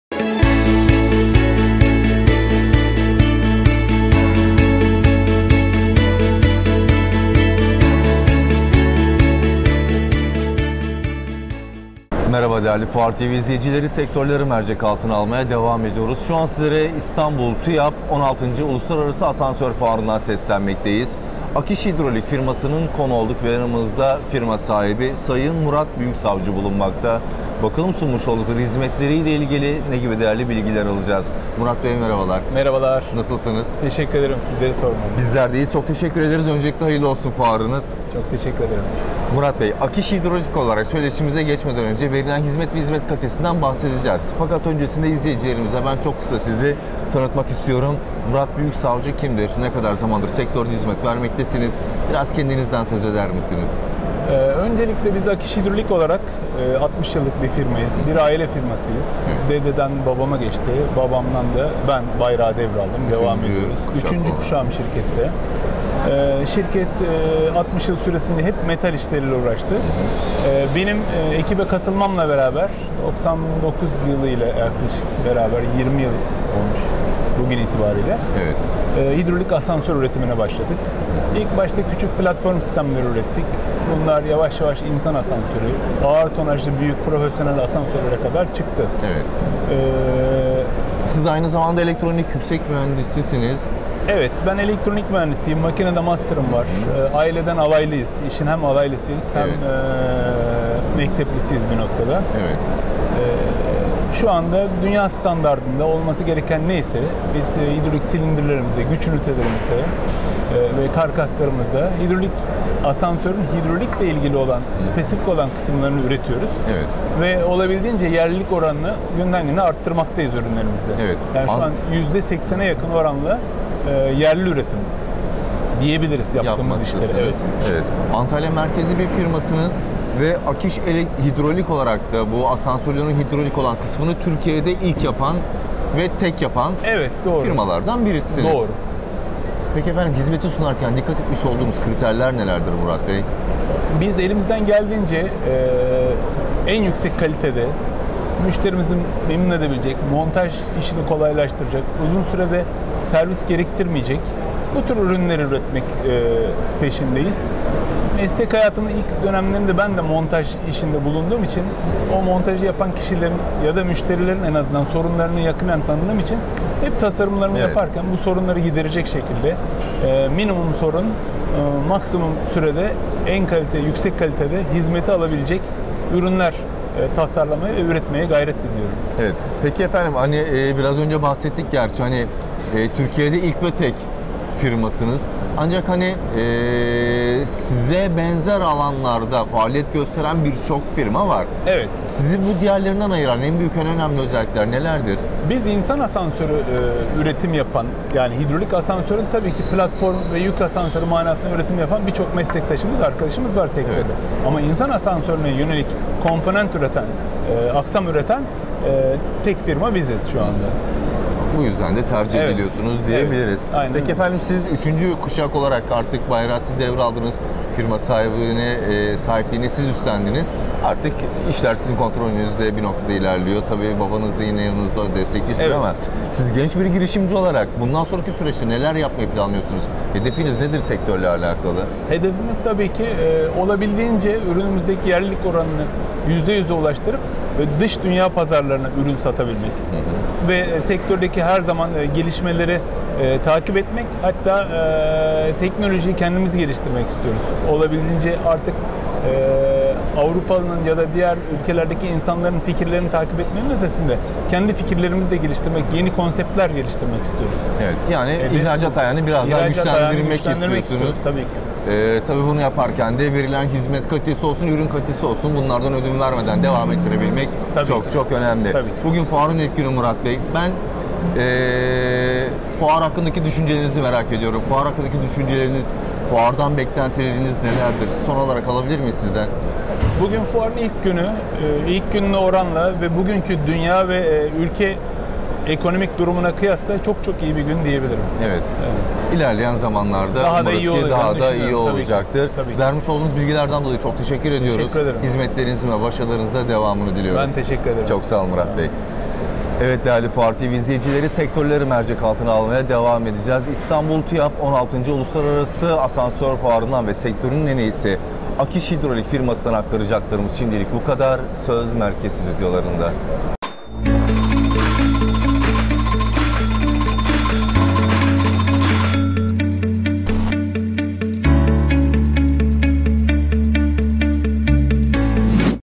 AKİŞ HİDROLİK - FUAR TV
Antalya’da faaliyetlerine devam eden sektörün önde gelen isimlerinden  “AKİŞ HİDROLİK” firmasının yetkilisi Murat Büyüksavcı,  firmanın standında muhabirimiz Cemal Yergin'in sorularını yanıtladı.